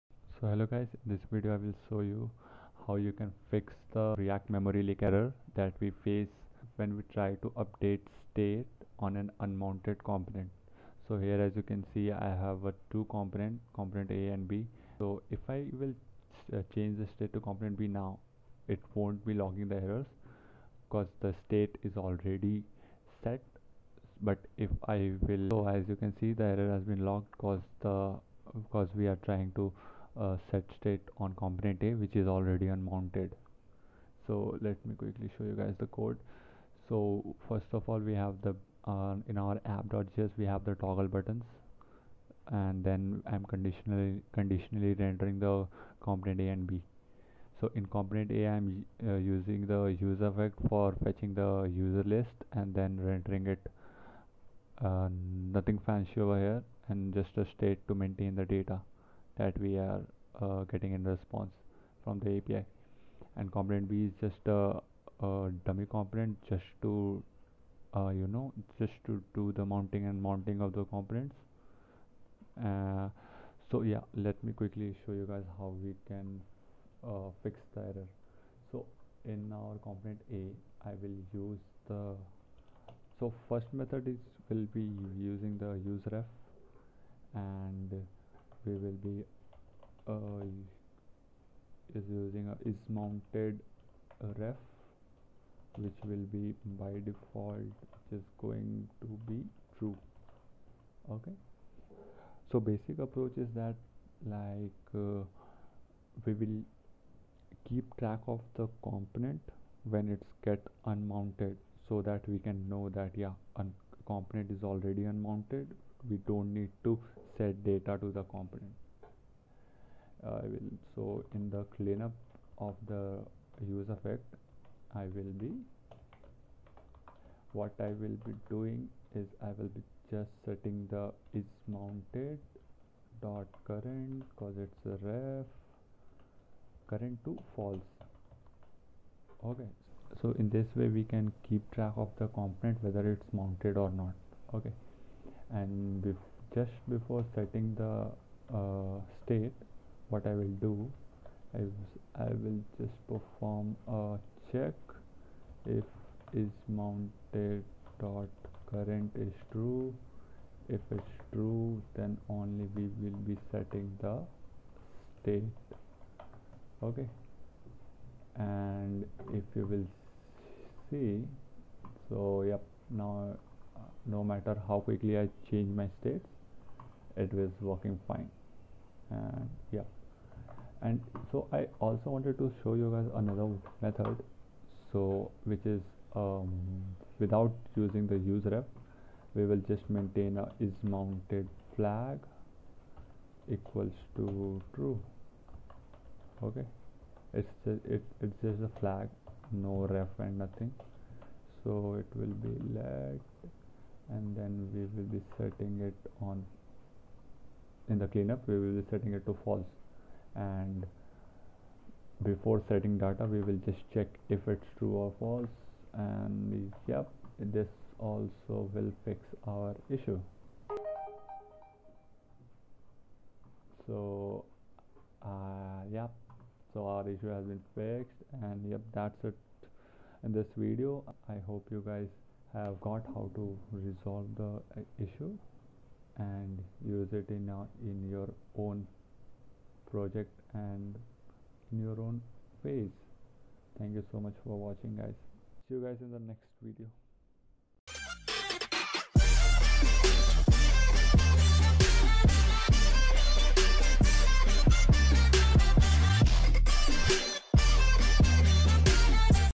How to fix React Memory Leak error in React App ? || 2 solutions under 5 mins || React Tutorials
In this video I have demonstrated how one can fix the annoying react memory leak error under 5 mins.
Two solutions your decision which one you prefer to go forward with.